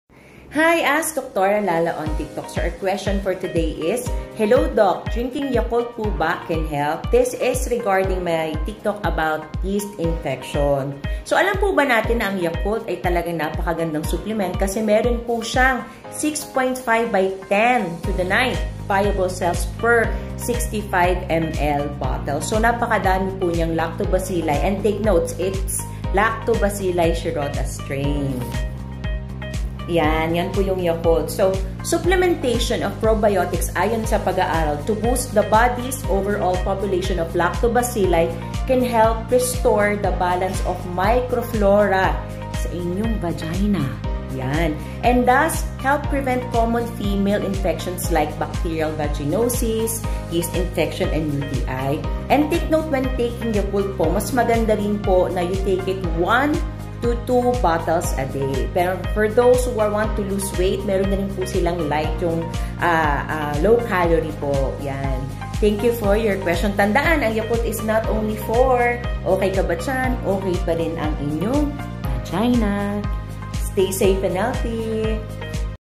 Yakult and yeast infection 😉
Supplementation with probiotics like Yakult may hel boost the body's overall population of lactobacilli can help restore the balance of microflora in the vagina and thus help prevent common female problems such as bacterial vaginosis, yeast infection, and urinary tract infection. @yakult